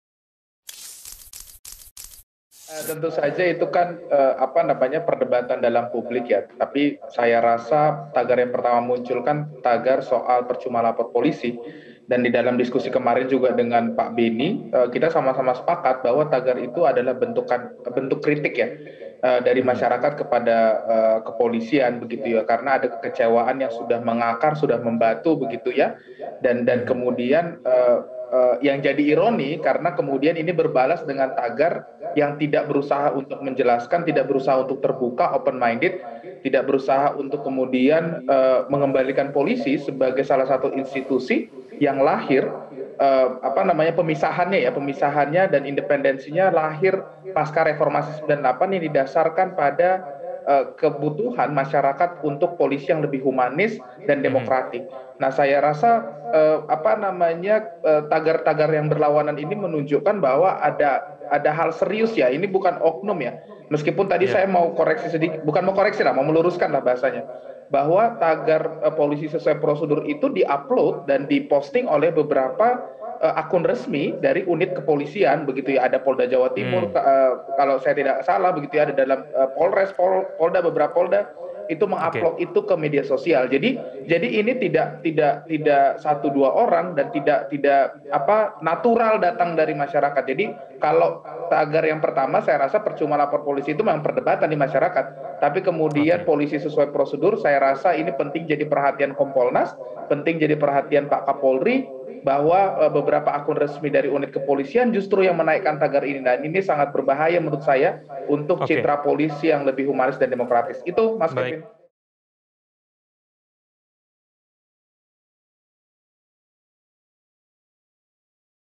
PolisiSesuaiProsedur Dinilai Berbahaya Bagi Citra Polri
Direktur Eksekutif ICJR Erasmus Napitupulu menilai PercumaLaporPolisi merupakan kritik dari masyarakat kepada kepolisian. Karena ada kekecewaan yang sudah mengakar di masyarakat pada kepolisian. Yang menjadi ironi, tagar ini berbalas dengan PolisiSesuaiProsedur. Tagar balasan itu dinilai tidak menyelesaikan masalah dan justru berbahaya bagi citra Polri. Sebab tagar diposting akun resmi kepolisian.